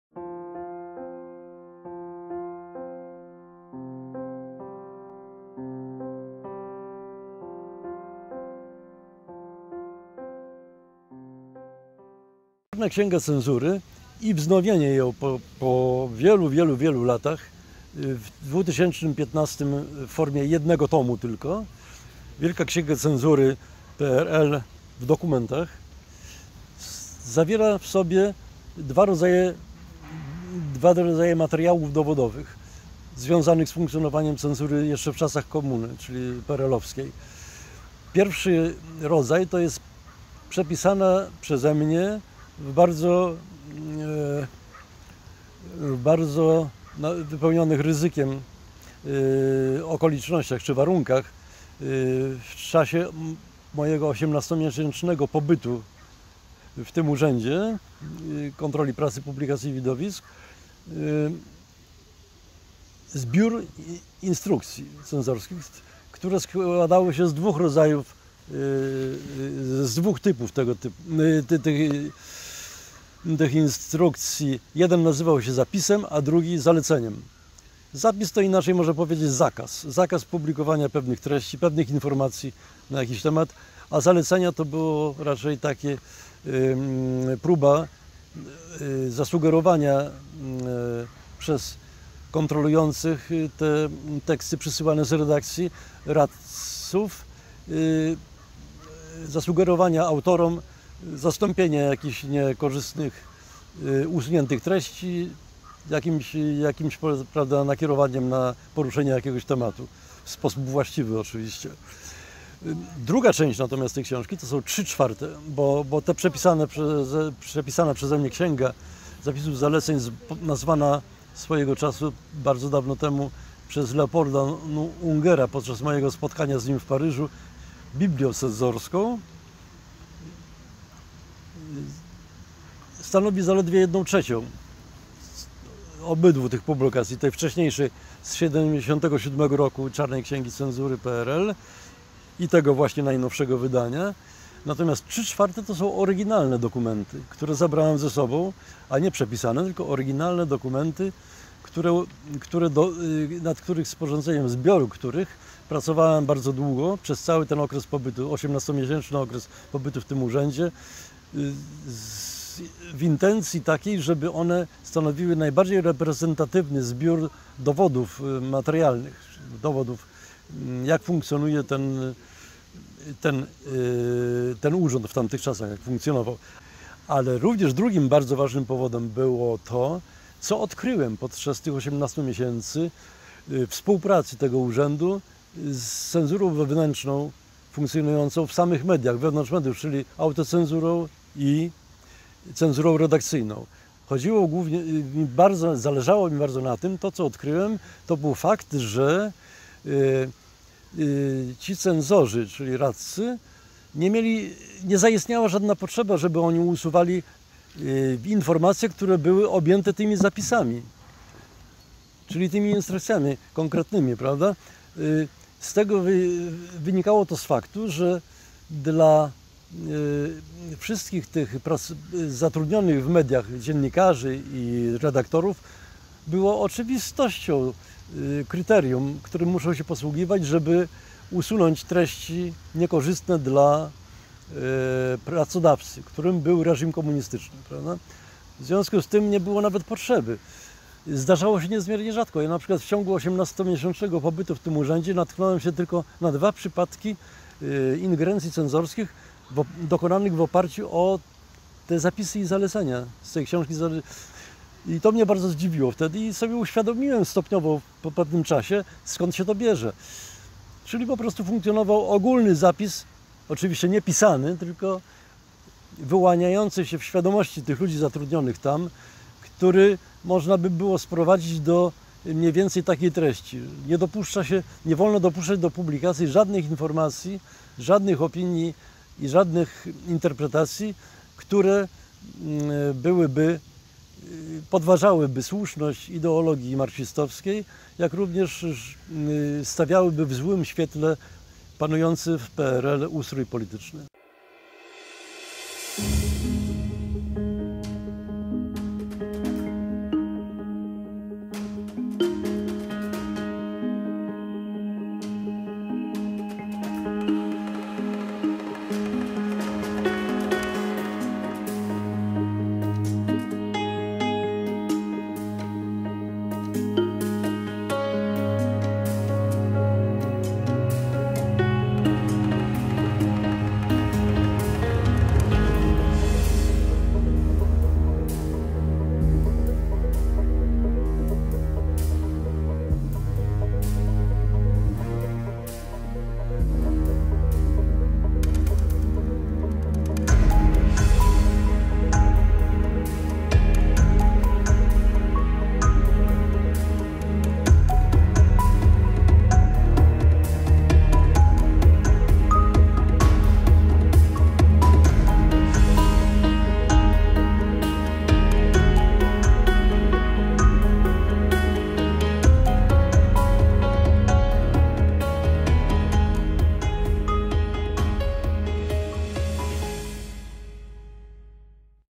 Tomasz Strzyżewski o książce "Wielka księga cenzury PRL w dokumentach"
Tomasz Strzyżewski opowiada o książce "Wielka księga cenzury PRL w dokumentach" i o tym, co odkrył podczas 18-miesięcznego okresu pracy w GUKPPiW. Czy cenzorzy i radcowie rzeczywiście byli niezbędni do funkcjonowania cenzury w PRL-u?